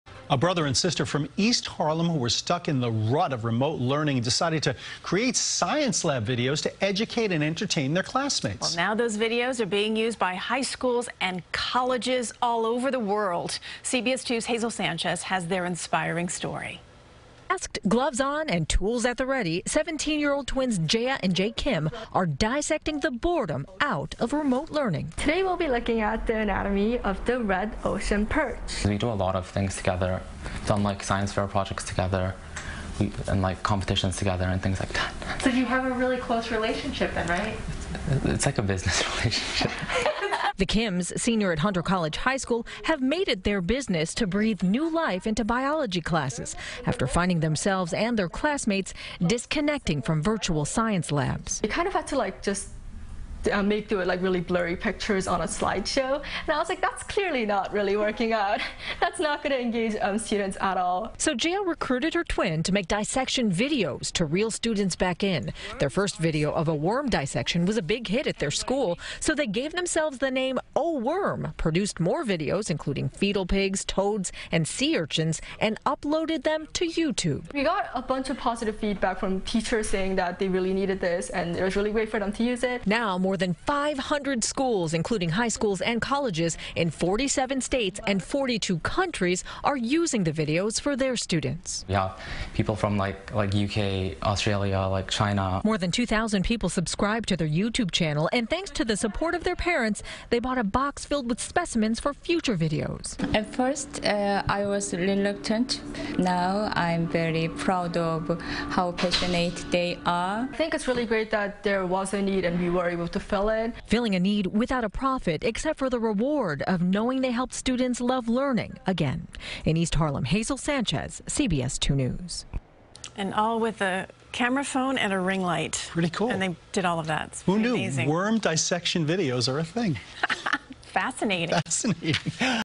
High Schools, Colleges Around The World Using Dissection Videos Made By East Harlem Siblings
A brother and sister from East Harlem who were stuck in the rut of remote learning decided to create science lab videos to educate and entertain their classmates. Now, the videos are being used by high schools and colleges around the world; CBS2's Hazel Sanchez reports.